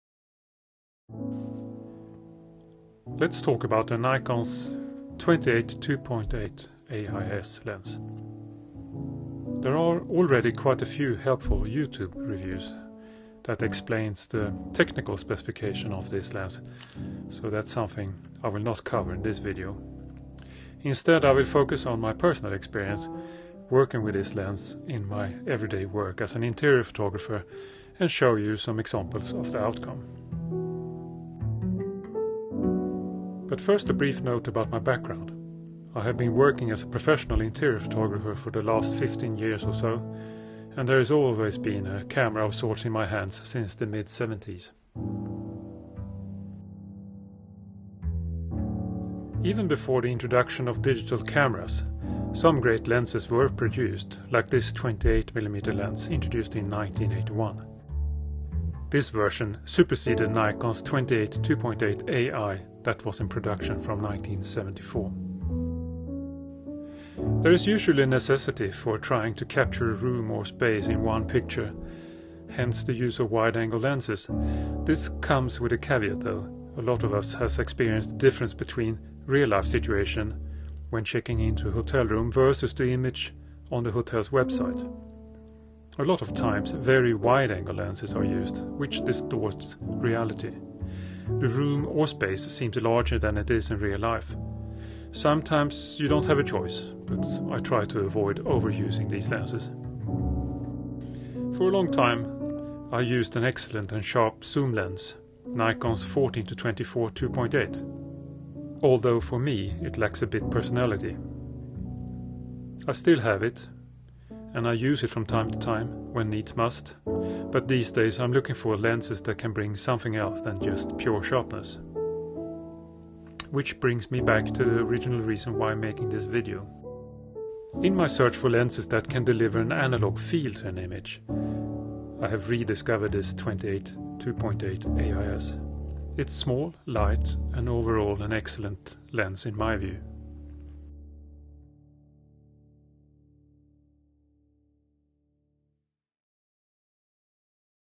The Nikon 28/2 8 AIS review
I work as a photographer, and this is my thoughts on this vintage lens.

28 mm works for me. Not too wide. Not too bleak. (Not a fence-sitter like close cousin 35, IMO).
Some would scream: 'It has no AUTO-FOCUS!!!  Well... Get over it! You don't need it! Henri Cartier-Bresson managed.

But, what about the aperture 2.8 you'll say? Isn't that a bit old-school? Yes, but in a good way. When shooting an interior, I very seldom need a 'fast' lens.
With the camera on a tripod, I usually stop down this lens between 8 or 11.

OK. So. If your attention span has reached its limit by now, my advice is: Just get one! You will not regret it. And if you have one, do not even think about selling it!

I will stop ranting now.

P.s.
Click 'thumbs-up' if you liked this video!